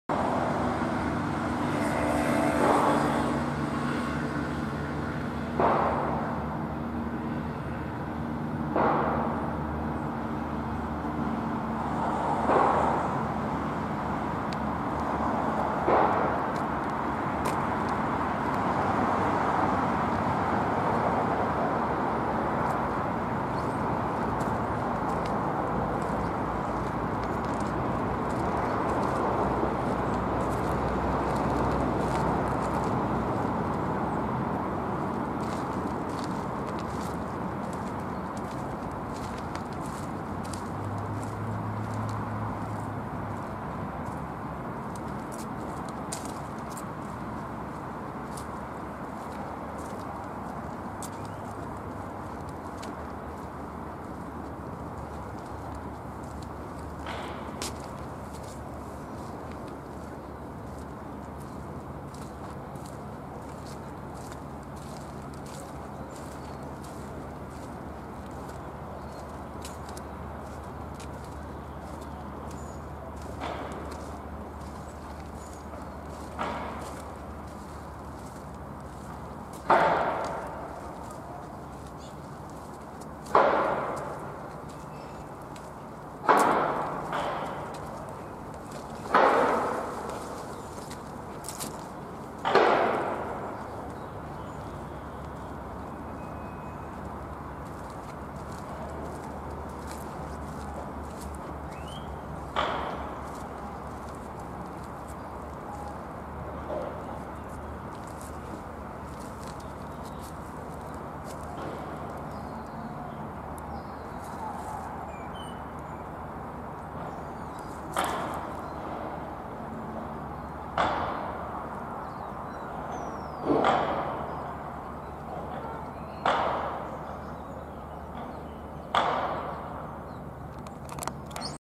Botanic Gardens around Christchurch Hospital #4 (2016-07-06-14:08)
Walking from the Riccarton Ave gates to North Hagley Park (near the Christchurch Public Hospital) to follow Addington Brook. Recorded 2016, 6th of July at 2:08pm.